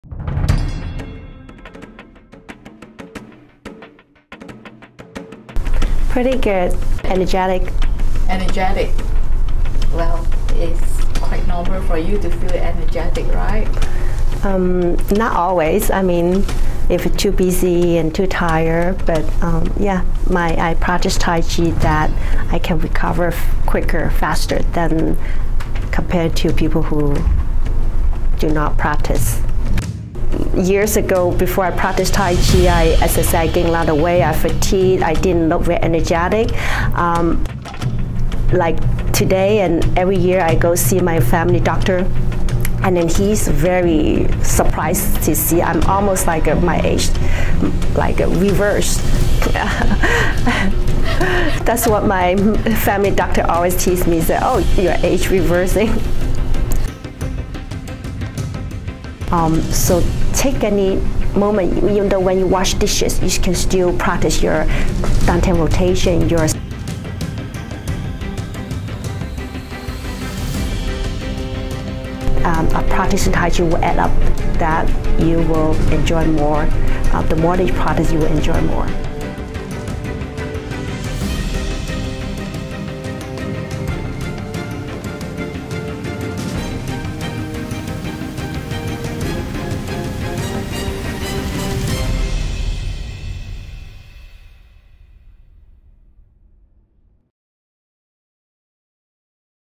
Energetics- An Art of Healing in Traditional Chinese Medicine
By practicing Tai Chi (Taijiquan) on a daily basis, Donna Tse maintains high energy levels throughout the day, and recovers quickly from exhausting daily schedules.  Her doctor is amazed at the anti-aging effects of Taichi, and comments that her age seems to be reversing!

Healing through rhythmic breathing and body movement, energetics is one of the five branches in Traditional Chinese Medicine. Donna teaches Tai Chi at Five Branches University based in San Jose, CA.